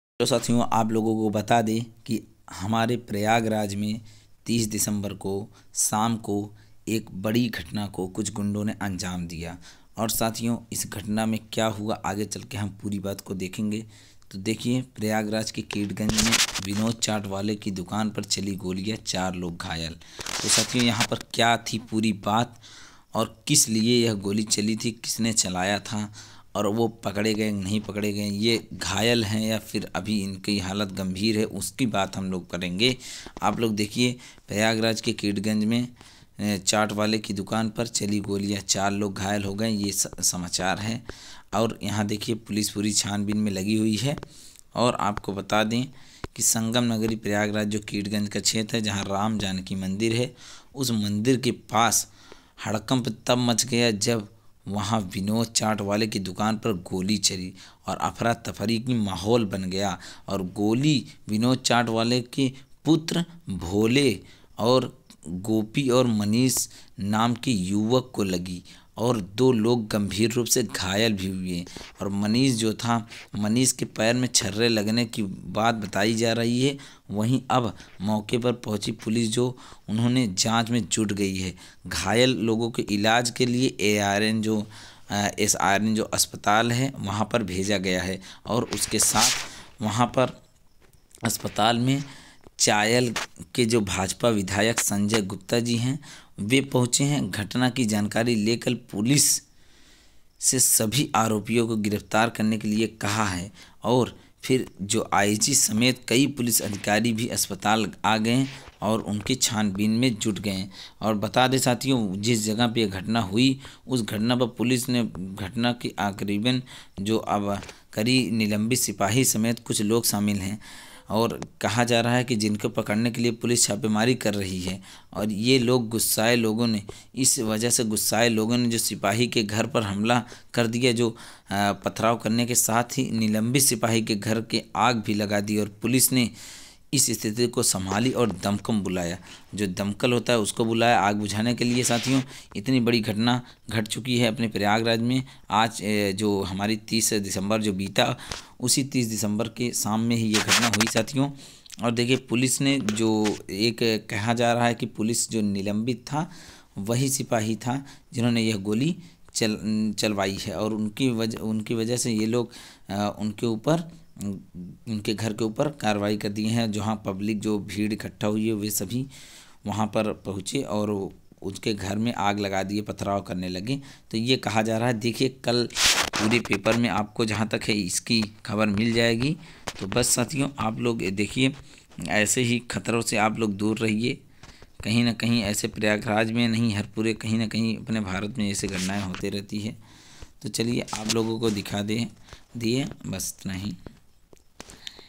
प्रयागराज के की‌डगंज‌ राम जानकी मंदिर के पास विनोद चाट वाले की दुकान पर चली गोलियां, चार लोग घायल।
30 दिसंबर को प्रयागराज में कीडगंज स्थान पर गोली चली गोली चलने से 4 व्यक्ति घायल हुए पूरी जानकारी आपको इस वीडियो में मिल जाएगी।

If you don't wish to miss any updates and or
 latest videos about

Mr Sp Point ☝️